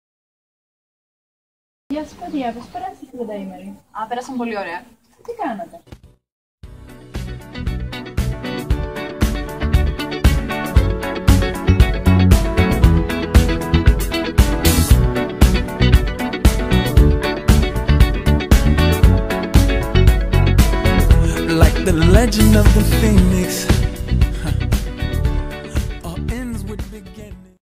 Video Kalamata τελικό 30''
Από την ιστοσελίδα του δήμου Καλαμάτας:
Το 2ο βραβείο δόθηκε σε συμμετοχή από το Λύκειο Αρφαρών, που ετοίμασαν οι μαθητές της B' Λυκείου: Ζαλαχώρης Κωνσταντίνος, Κώτσαρης Ηλίας, Ρηγοπούλου Βασιλική – Ερατώ, Τζαβάρα Ευαγγελία – Γεωργία, Τουρνά Δήμητρα.
Συγχαρητήρια!